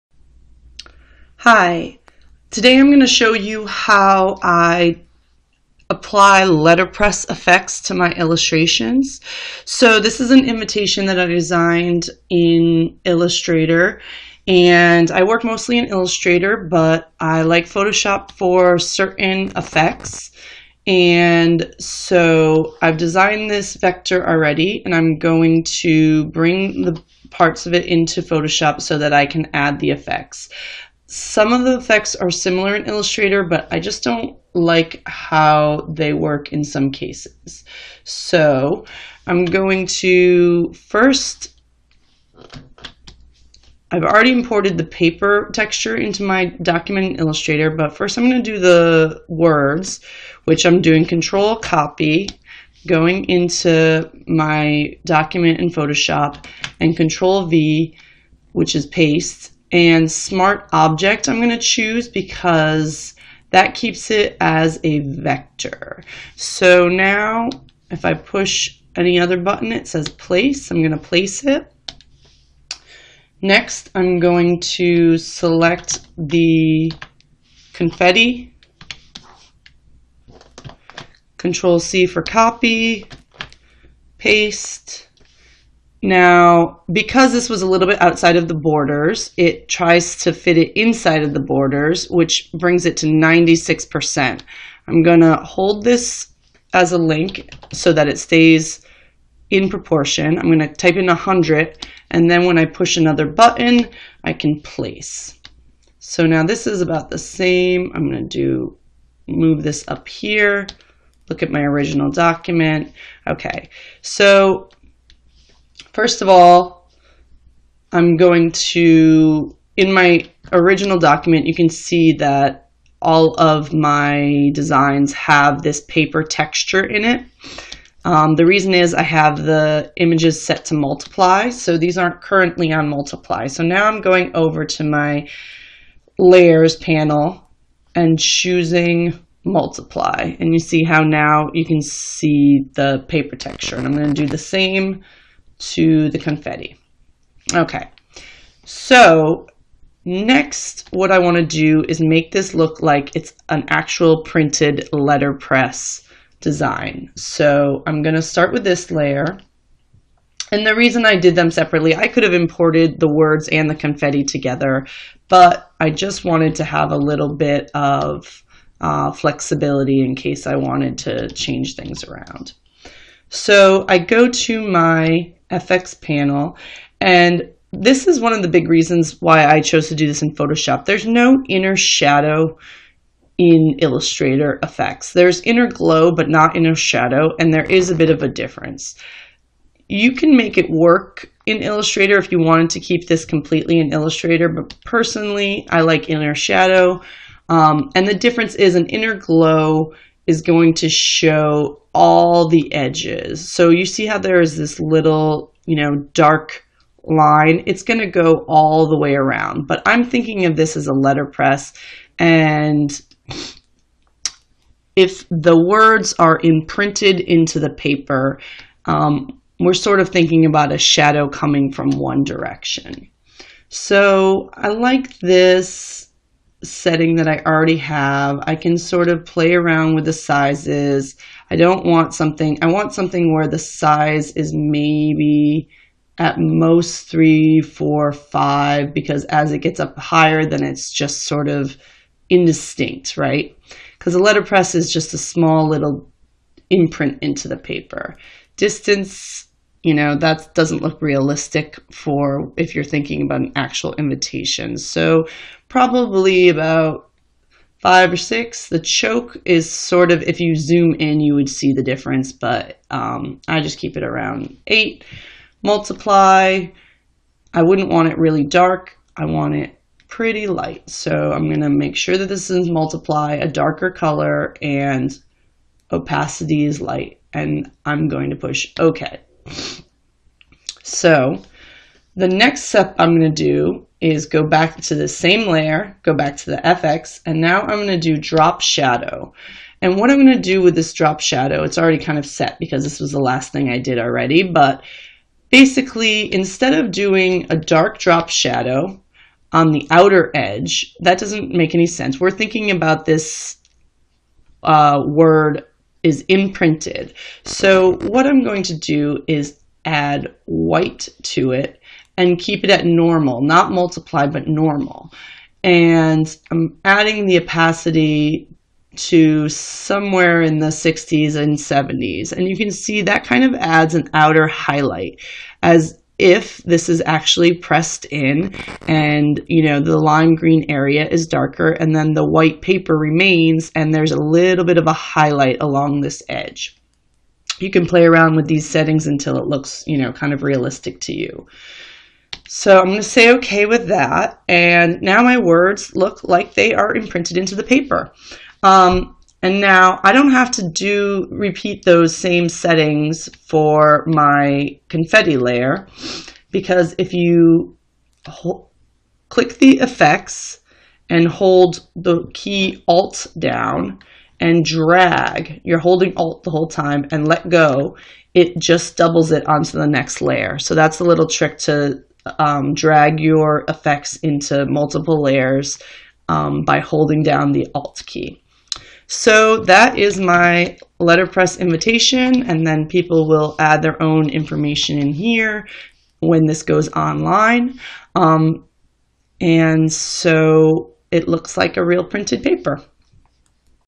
Letterpress Effect in Photoshop
Learn how to quickly create a faux letterpress effect in Adobe Photoshop.